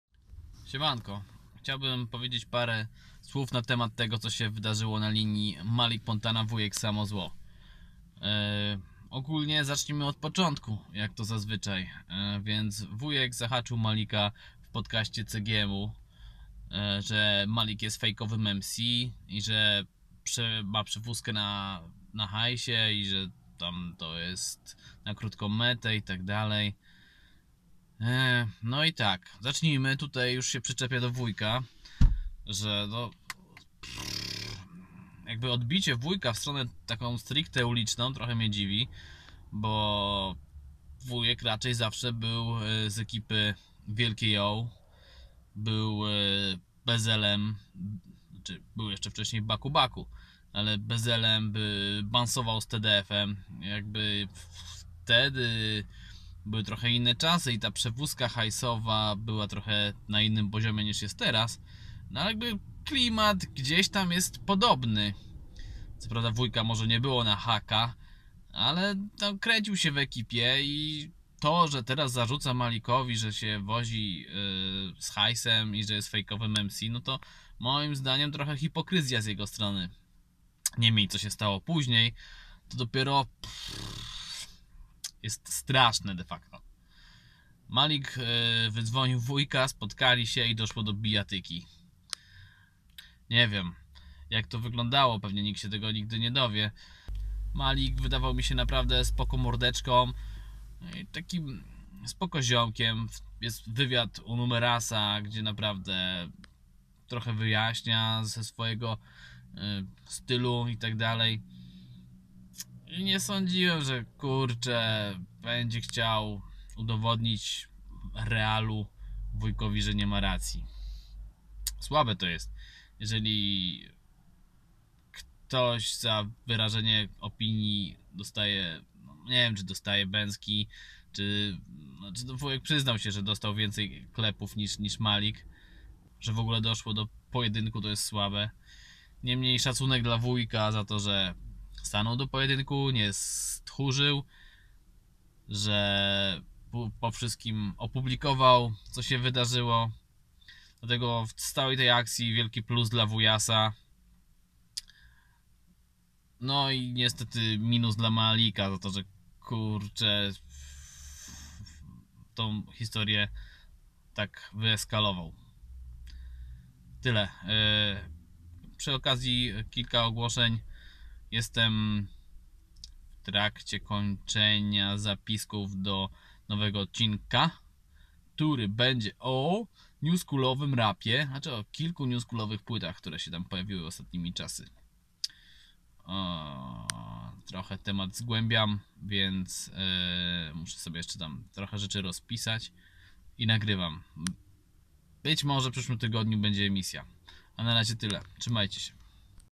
Wujek Samo Zło Vs Malik Montana
W filmie moje zdanie na temat akcji na Lini WSZ Malik